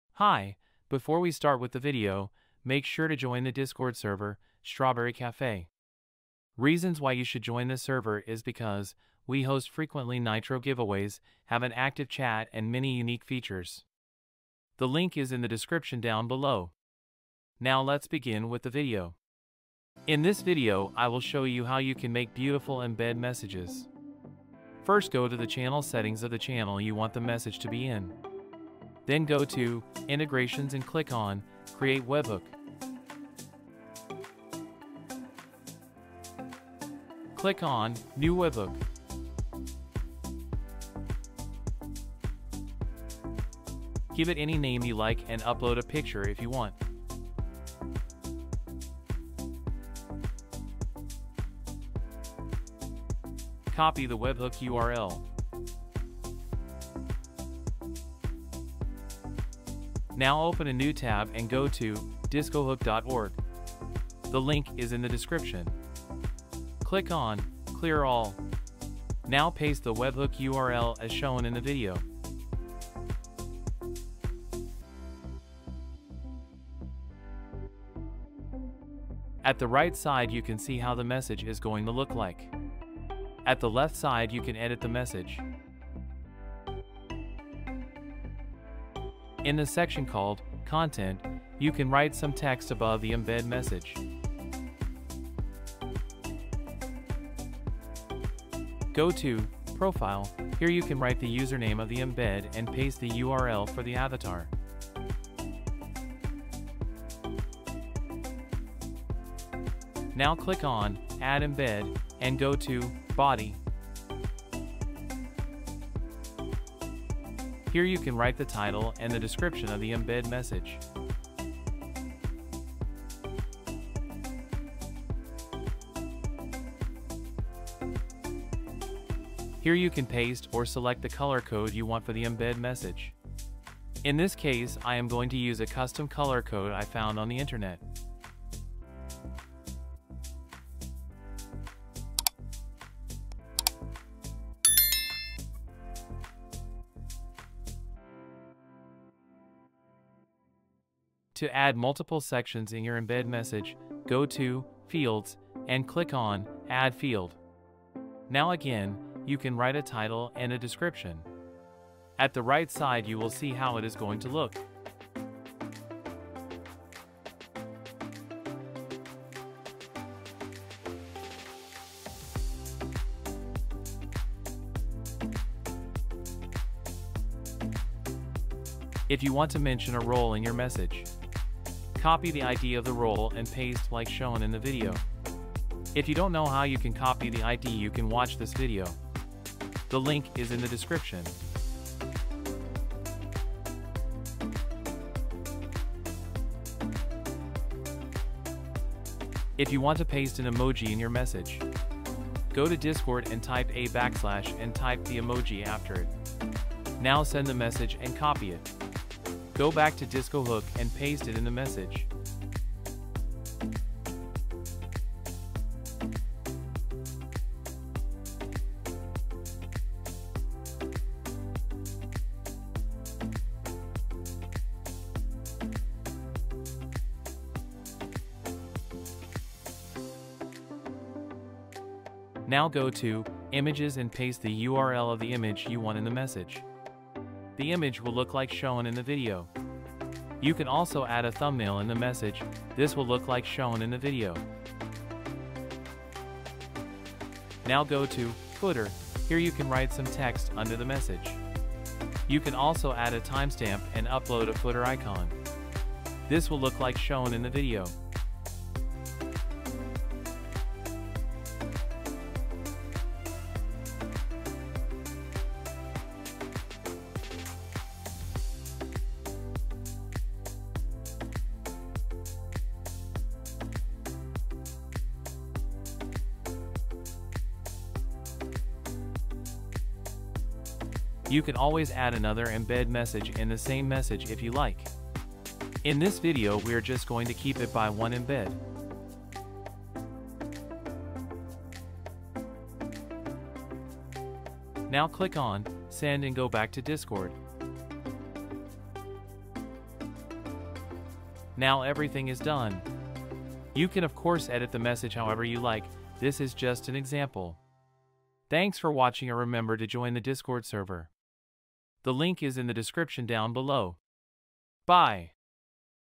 How to make embed messages on discord | 2023
What is a discord embed message?

A Discord embed message is a powerful tool for displaying rich media within a message on the Discord platform. With embed messages, users can add a variety of media, such as images, videos, links, and formatted text, to their messages in a visually appealing way.

An embed message consists of a customizable box or container that can display a title, description, image, fields, footer, and other metadata. Users can easily customize the appearance of the embed message using Markdown syntax or Discord's built-in embed builder tool.

Embed messages are widely used in Discord servers for a range of purposes, including announcements, event invites, and other important information that requires more formatting and visual elements than a regular message. Additionally, they can be used for displaying information from external sources, such as Twitch streams, Twitter feeds, or Reddit posts.

By following our guide, you'll learn how to use Discord embed messages to their fullest potential, making it easy to create visually appealing messages that stand out on the platform.